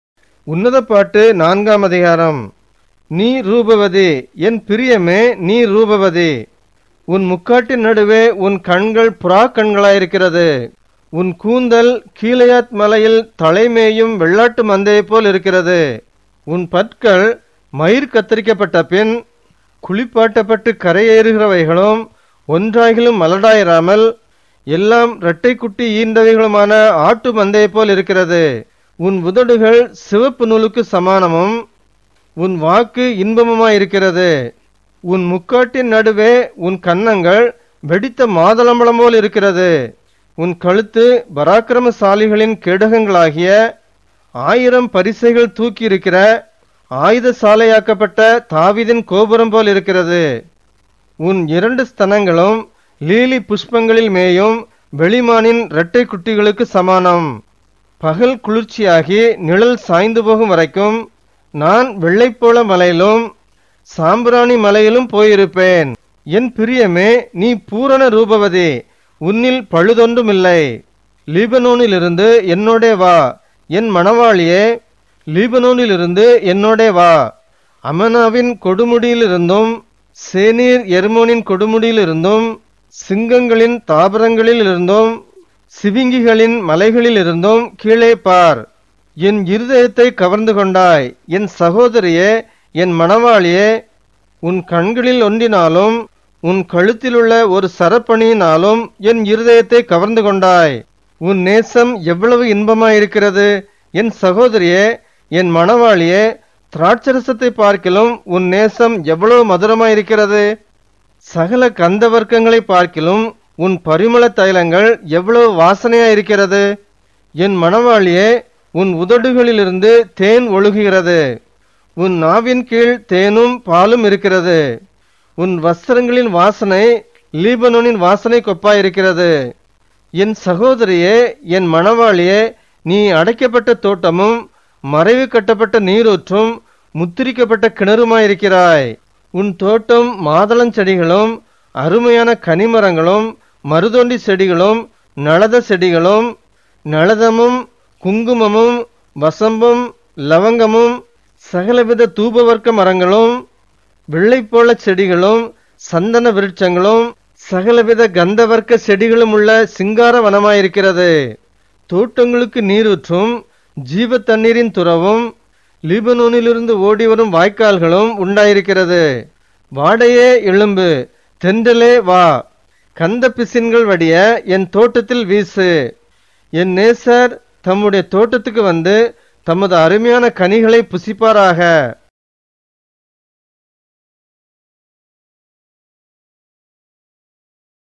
22 Song of Solomon 4, உன்னதப்பாட்டு அதிகாரம் 4, தமிழில் வேத வாசிப்பு - தமிழ் & ஆங்கில வசனங்களுடன்.
Song of Solomon 4, Tamil Bible Reading with Verses in Tamil & English.